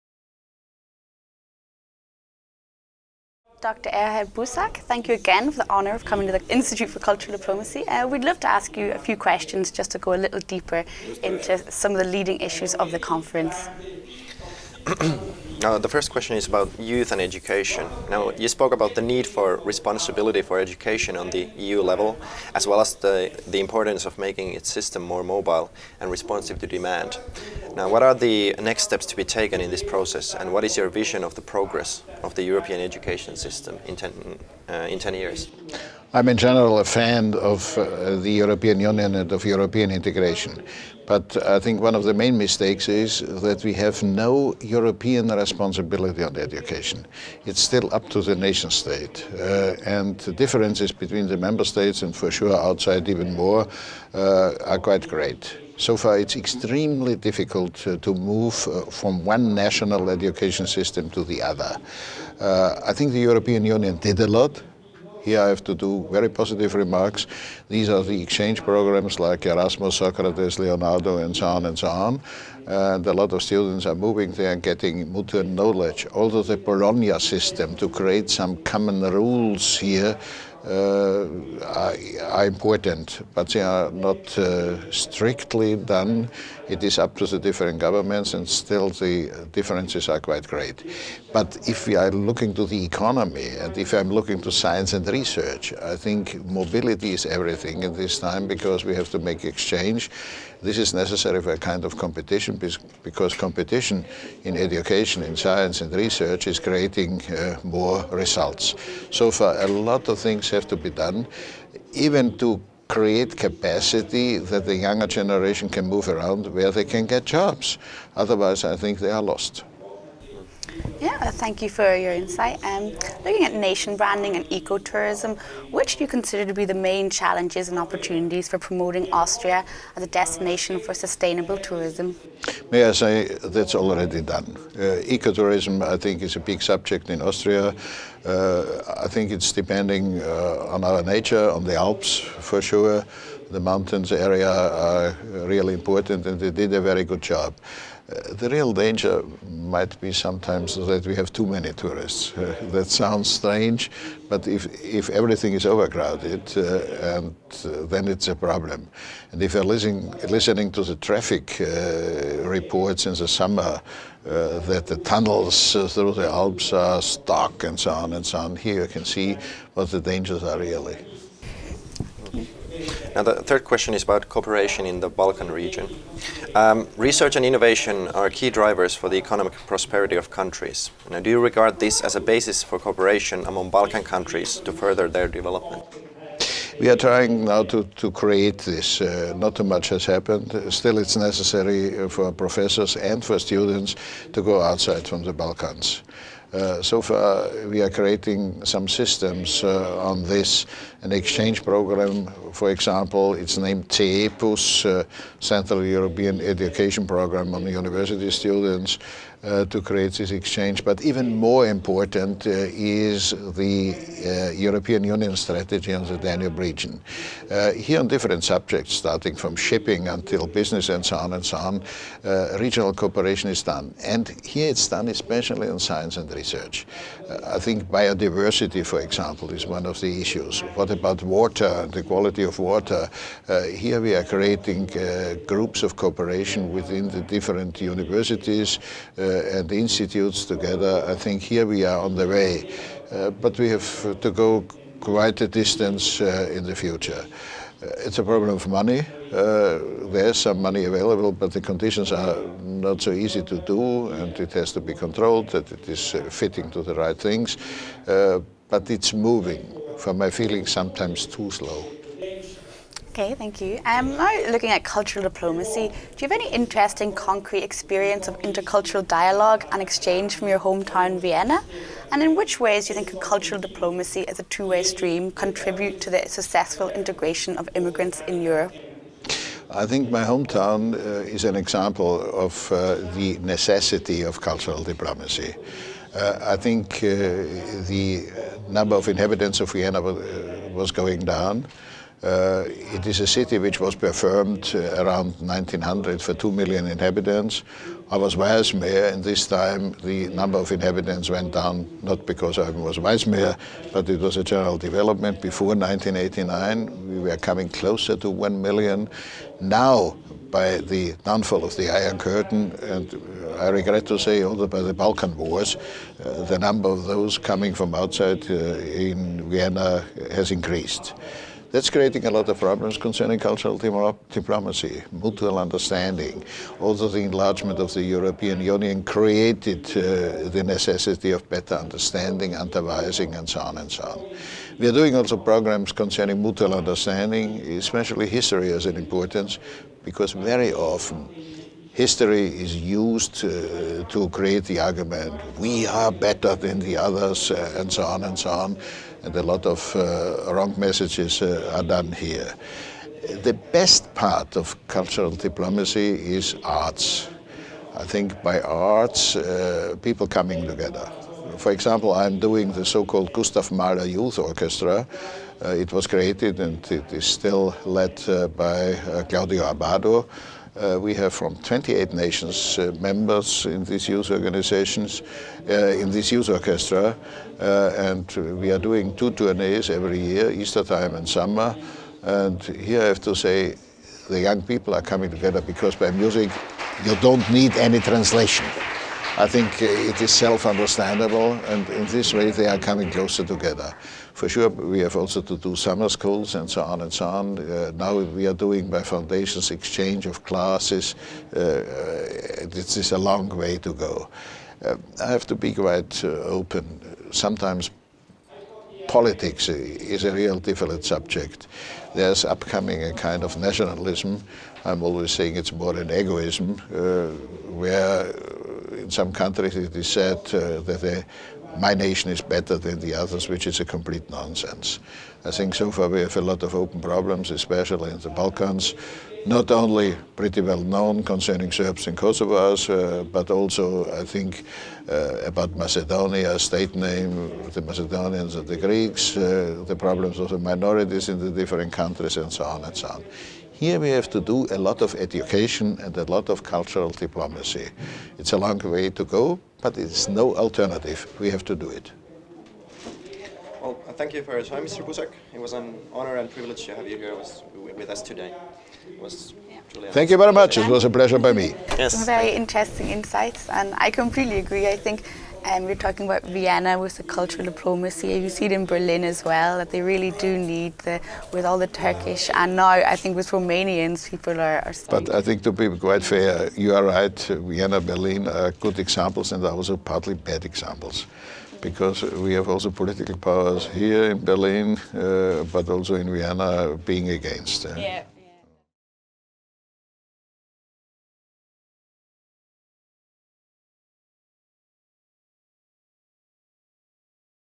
Erhard Busek, Former Vice-Chancellor of Austria
An Interview with the Hon. Dr. Erhard Busek , Former Vice-Chancellor of Austria; Former Minister for Science and Research of Austria.

The Interview was made by Lena Johanna Campbell and Olli Pentti Antero Suominen (MA Students, Center for Cultural Diplomacy Studies)

The Berlin International Economics Congress 2013
(Berlin, March 6th-10th 2013)

Center for Cultural Diplomacy Studies Publication
Institute for Cultural Diplomacy